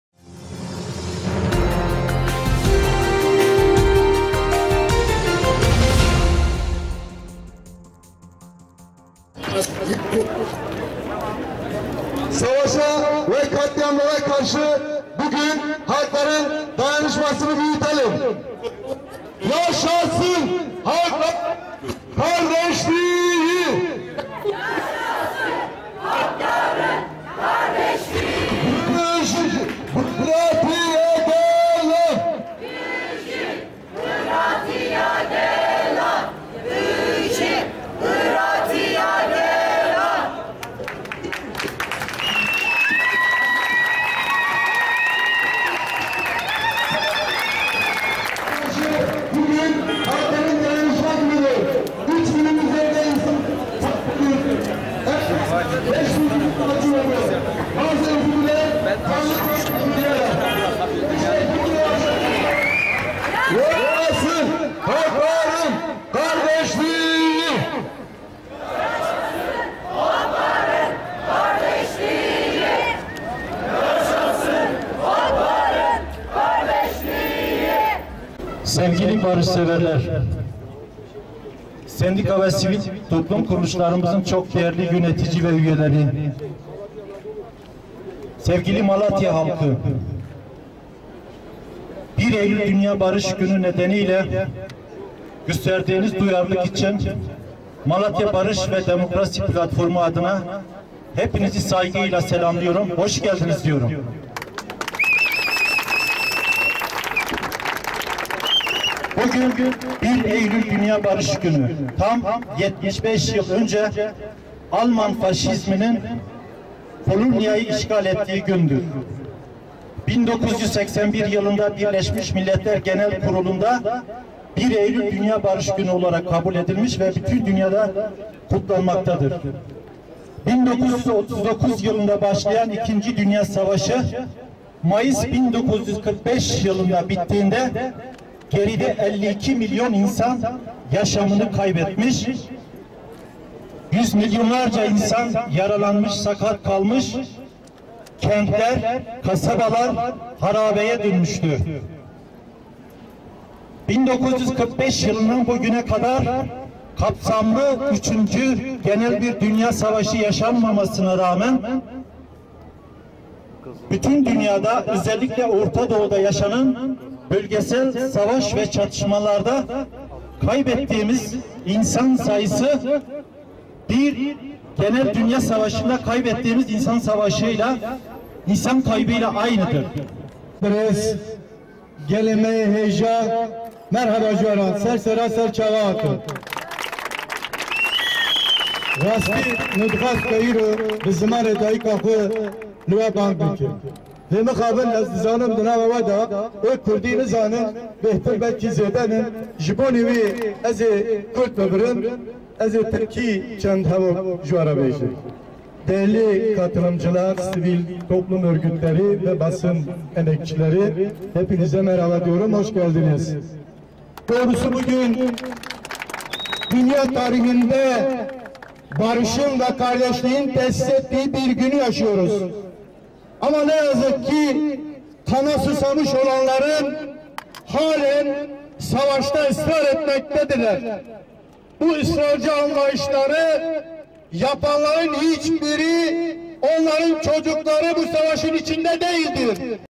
Malatya’da 1 Eylül Yürüyüşü
- Malatya’da 1 Eylül Yürüyüşü - Malatya'da 1 Eylül Dünya Barış Günü sebebi ile Malatya'da Emek ve Demokrasi Platformu öncülüğünde bir yürüyüş yapıldı. Soykan Parkı'na kadar yüründü